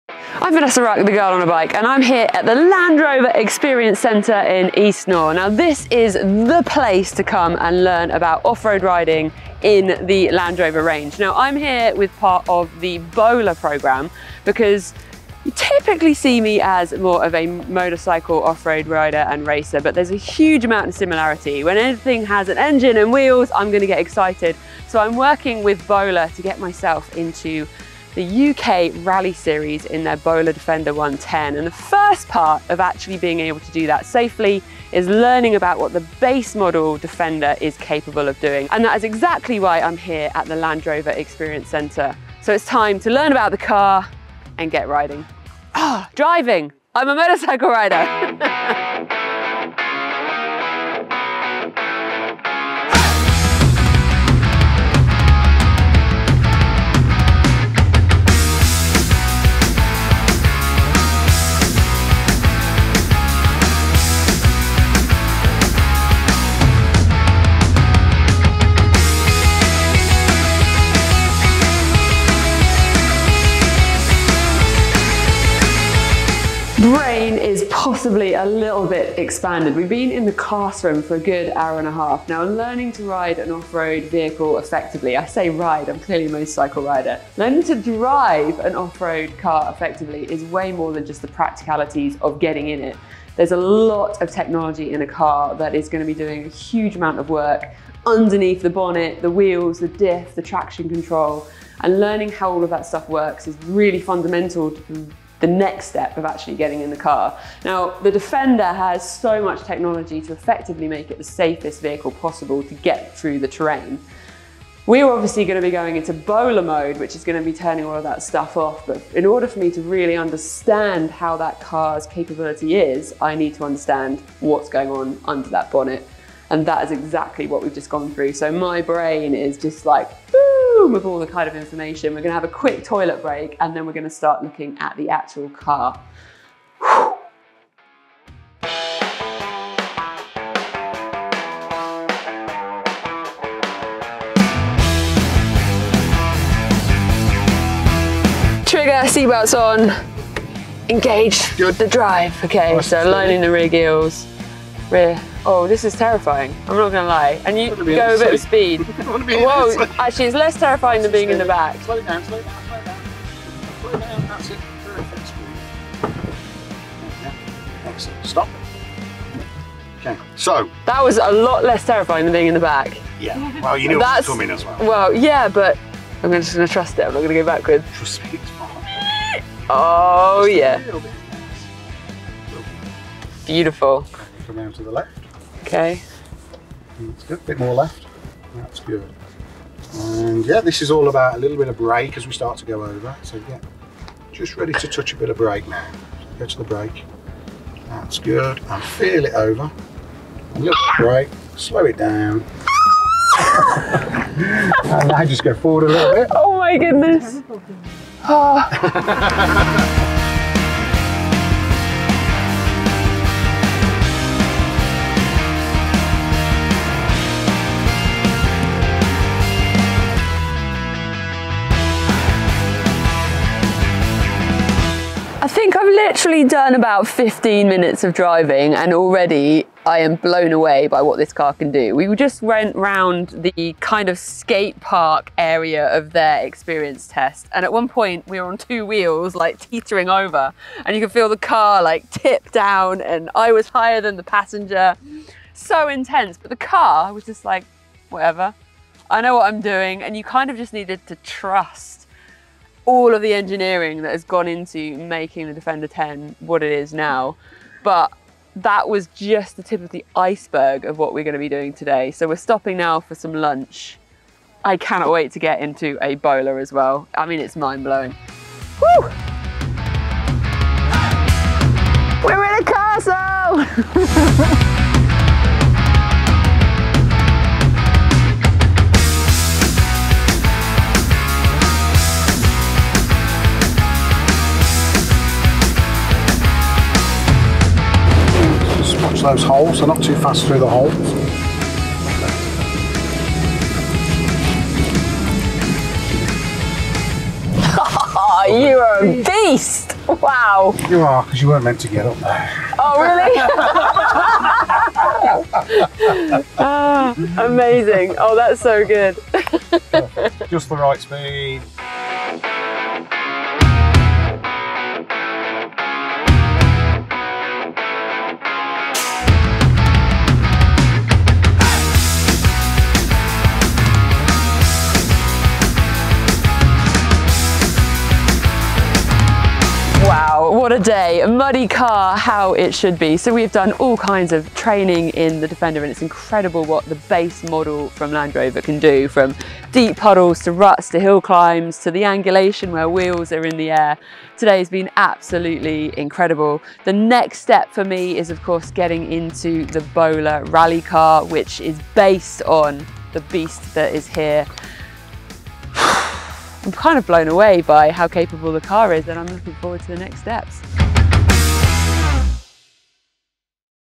Brand Ambassador Vanessa Ruck undergoes Bowler Training | Ep 1
In this exciting video, Bowler Brand Ambassador Vanessa Ruck undergoes Bowler training in a Defender 110. Watch as she learns the ins and outs of driving this impressive vehicle and hones her skills on a challenging course. With expert guidance from Bowler's experienced team, Vanessa navigates through rugged terrain, steep climbs, and tricky descents, pushing the limits of the Defender 110 and showcasing its impressive capabilities.